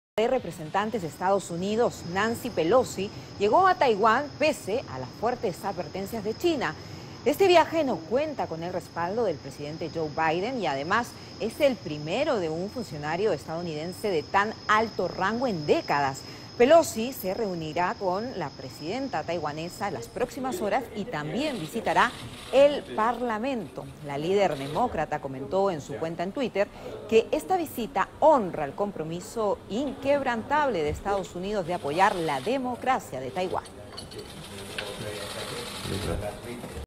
Nancy Pelosi aterriza en Taiwán pese a advertencias y aviva tensiones entre EE.UU. y China
La presidenta de la Cámara de Representantes de Estados Unidos Nancy Pelosi ha ignorado las advertencias de China y a los consejos del propio gobierno estadounidense, tras aterrizar hoy martes2 de agosto en Taiwán.

“La visita de nuestra delegación honra el compromiso inquebrantable de EE UU con la vibrante democracia taiwanesa”, escribió en su cuenta de Twitter la política demócrata pocos minutos después de aterrizar.

ACTUARÁN CON OPERACIONES MILITARES

Ante ello, el Ministerio de Relaciones Exteriores chino emitió un comunicado condenando dicha visita, la cual “envía un mensaje erróneo sobre la independencia de Taiwán a las fuerzas separatistas”.

En su misiva, la Cancillería reitera que “solo hay una China en el mundo, y Taiwán es parte inalienable de ella” y que “el único Gobierno legítimo es el de la República Popular China”.

El Ministerio de Defensa de China señaló que el ejército está en alerta máxima y lanzará "operaciones militares dirigidas" para contrarrestar la visita de Pelosi a Taiwan.

El país enfatiza que el culpable del aumento de las tensiones y los desafíos en el estrecho son las autoridades taiwanesas y estadounidenses por “cambiar constantemente el estatus quo”.